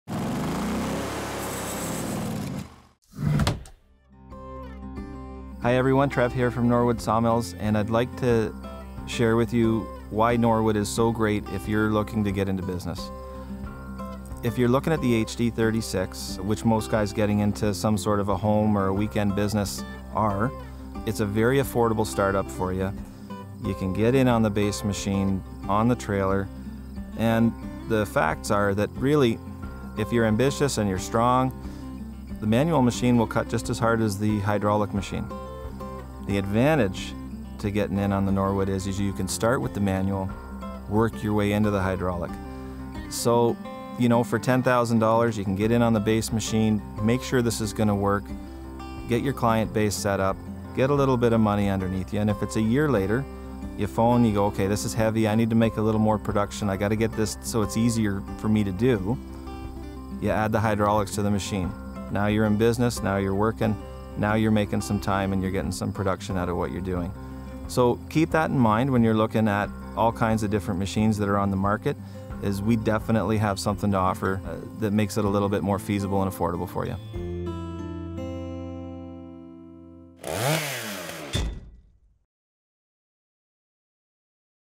SAWMILL TECH TIPS - Start Manual. Build to Fully-Hydraulic As Your Business Grows
If you want to use your saw for profit, consider what sawmill will best allow you to grow your business.

In this episode of SAWMILL TECH TIPS, Norwood pro-staff sawyer Trevor discusses how you can start very affordably with a manually-configured Norwood HD36.  Then, as your sawing business grows, you can add hydraulic and power functions to keep up with your more demanding sawing needs.

We welcome more questions about sawmilling and Norwood sawmills.